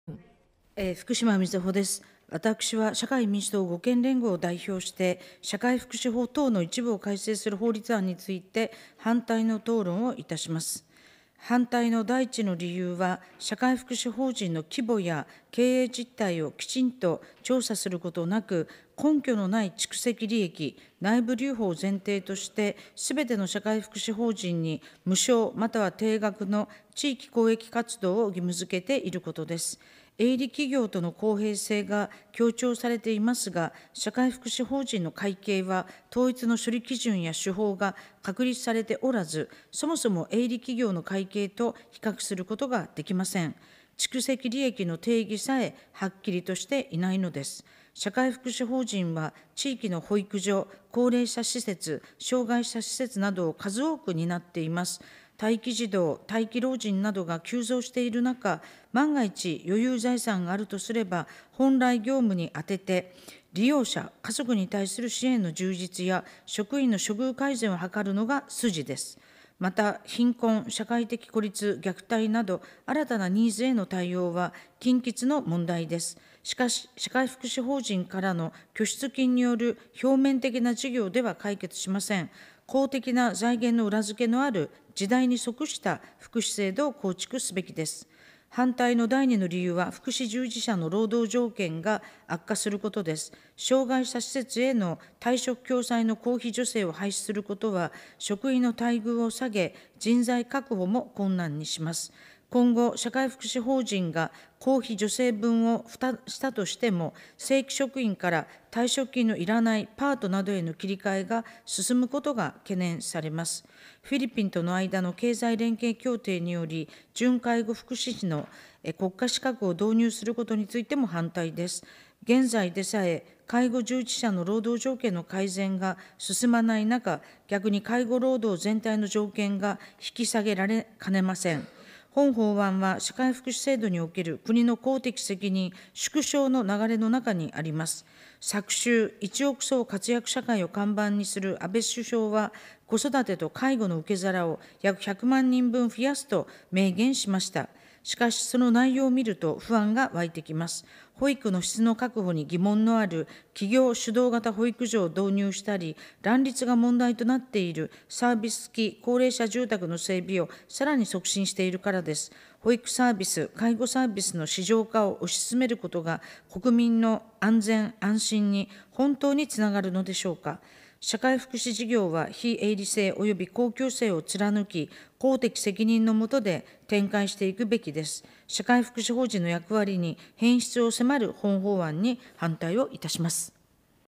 2016年3月17日 厚生労働委員会その2－反対討論 （福島みずほ国会質問シリーズ）
2016年3月17日 厚生労働委員会での福島みずほ国会質問動画。社会福祉法の一部改正案に対する反対討論です。